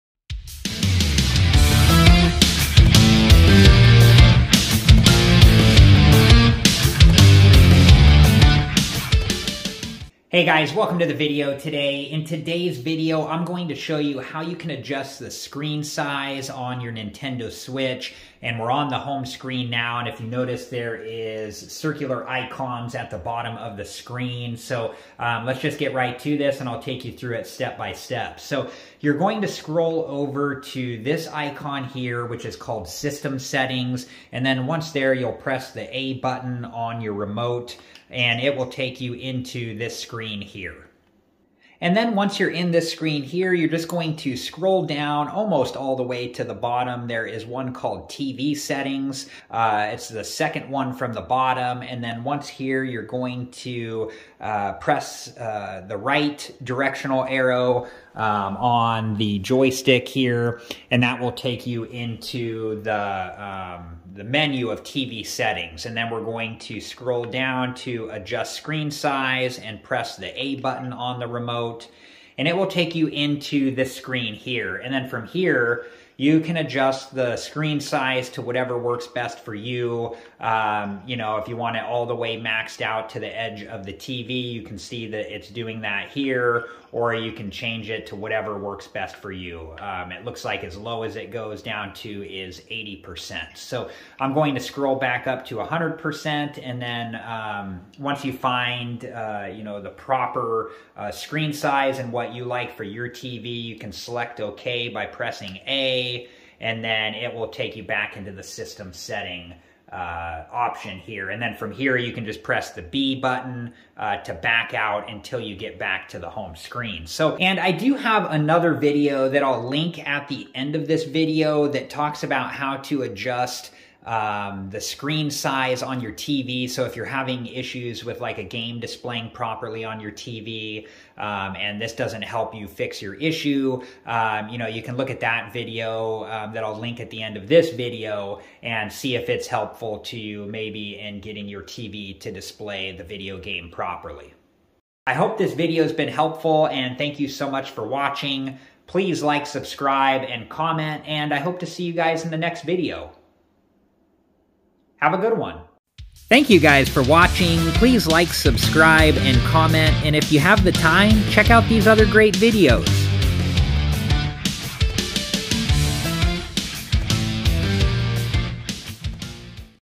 How To Adjust Screen Size On The Nintendo Switch
In this video I show you how to adjust the screen size on the Nintendo Switch. I take you through how to adjust the screen size step by step. You can use this option to fit the screen size to your TV or to adjust it to however you want it to be. This is something you want to look at if your game is not displaying properly. I also have a video showing how to adjust the screen size on your TV if this option doesn't resolve the problem. I will link that video at the end of this video, if your interested and have time please check that video out.